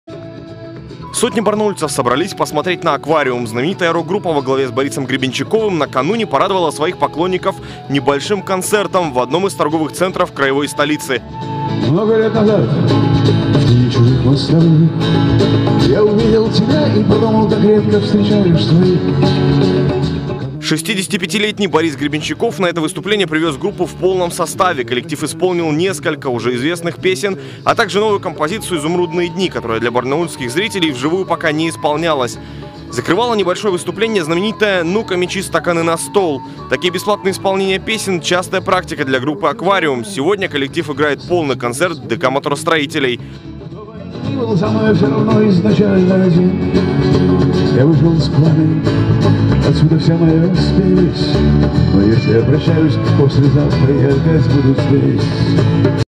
Легендарная группа «Аквариум» выступила в краевой столице
Аншлаг в торговом центре. Сотни горожан насладились выступлением Бориса Гребенщикова - бесплатно